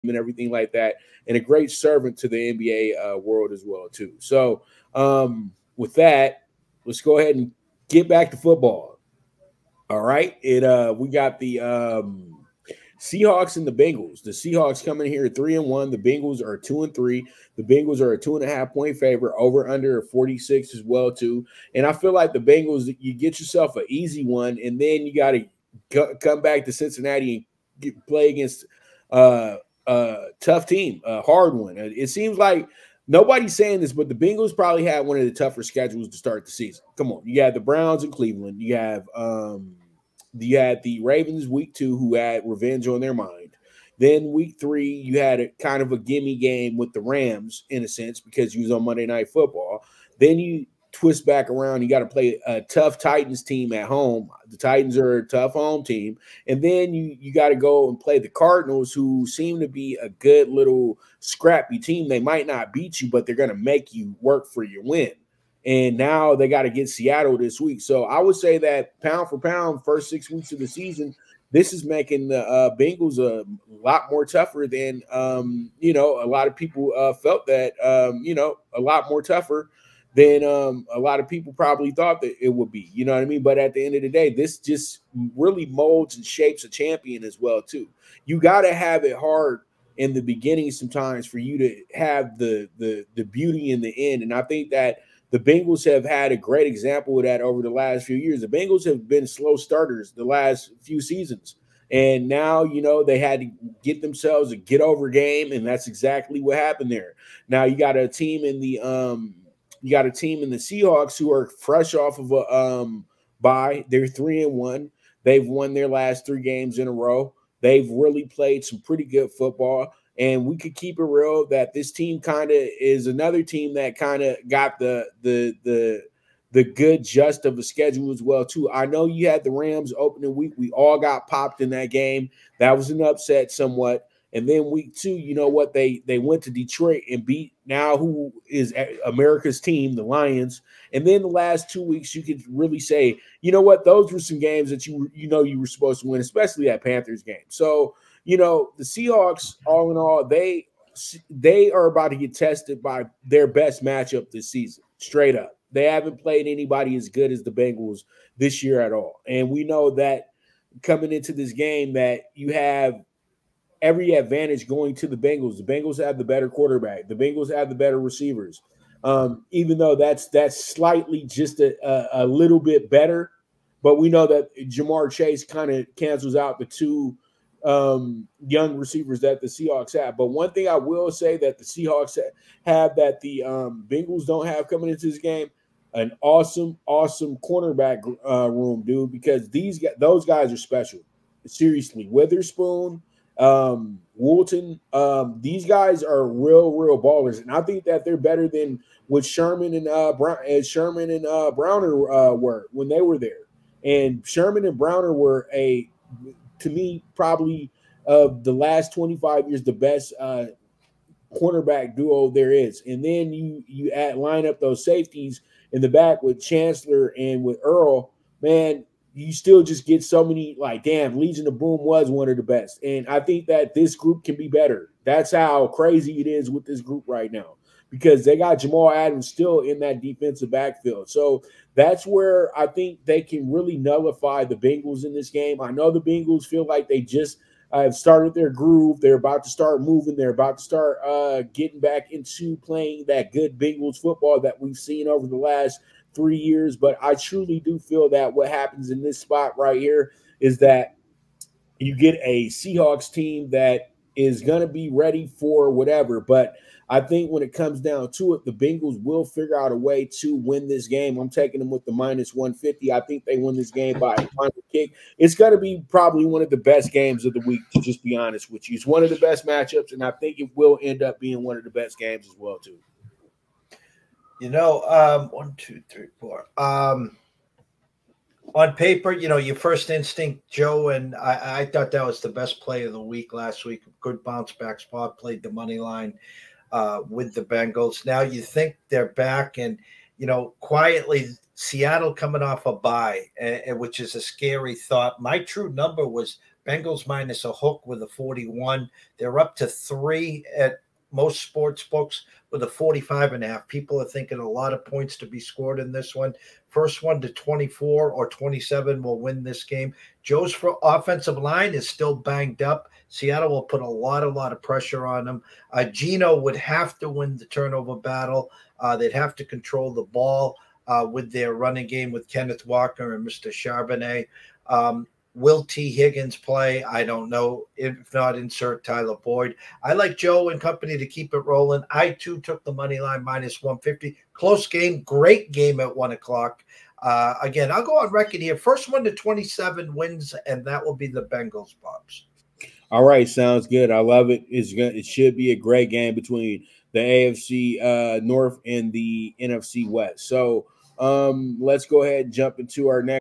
NFL BET EXCHANGE: Seahawks(3-1)@Bengals(2-3)(-2.5)O/U:46 GAME PREVIEW
This is a candidate for the Game of the Week! features 2 of the more exciting offenses in the NFL. Both with highly potent passing offenses. The guys breakdown the game and give the best bets to lay